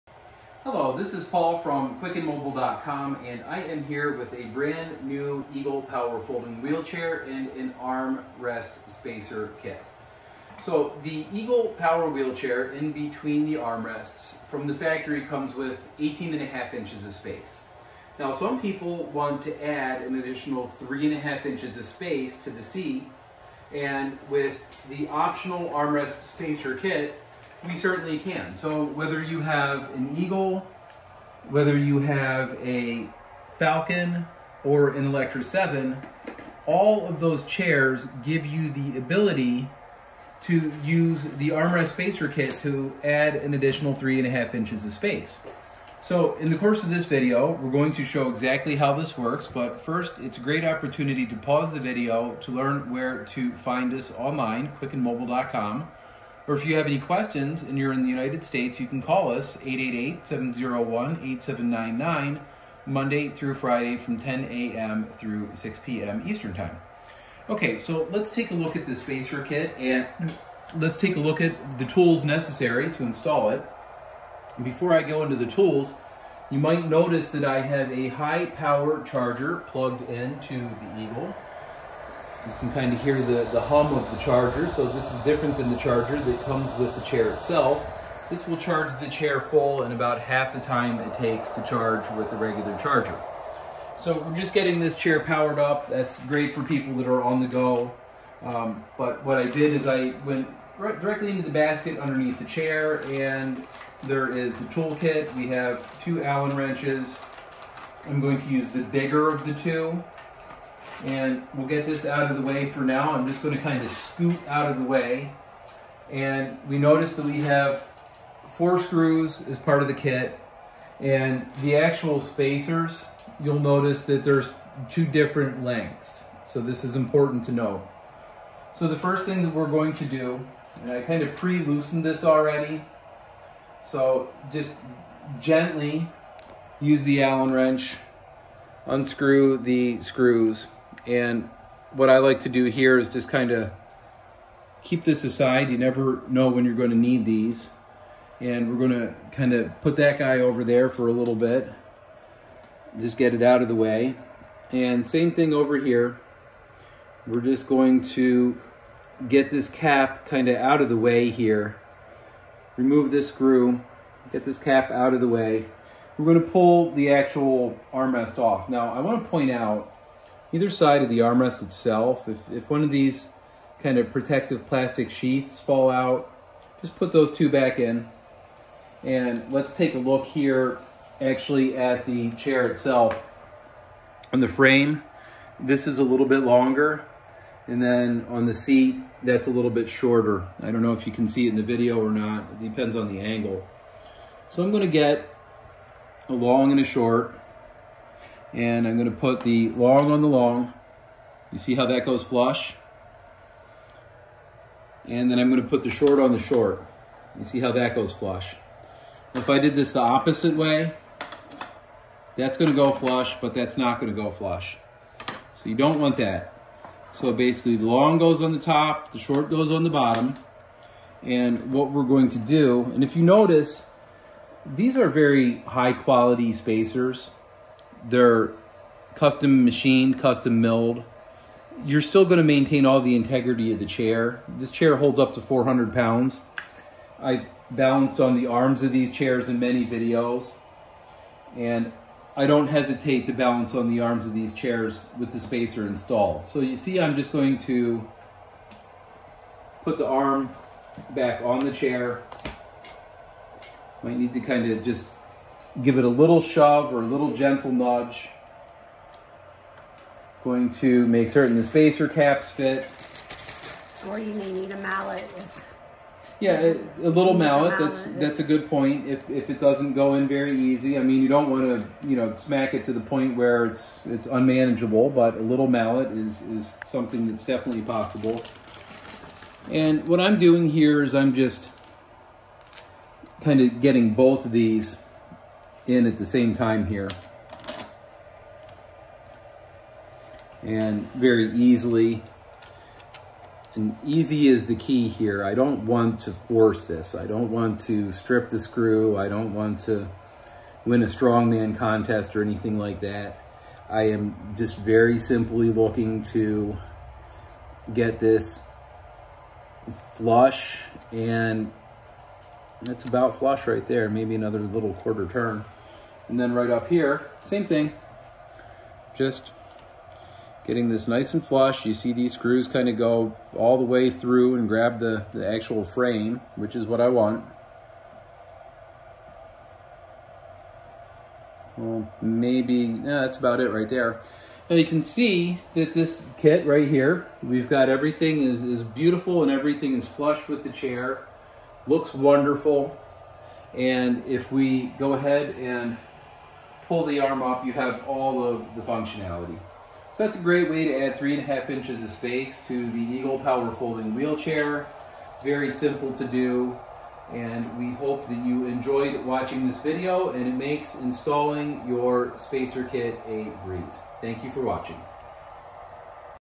How to Install The Armrest Spacer Kit for Eagle, Falcon and Electra7 Wheelchairs
This video shows how to install the updated armrest spacer kit on your Eagle, Falcon, or Electra7 power folding wheelchairs. For additional information, please visit: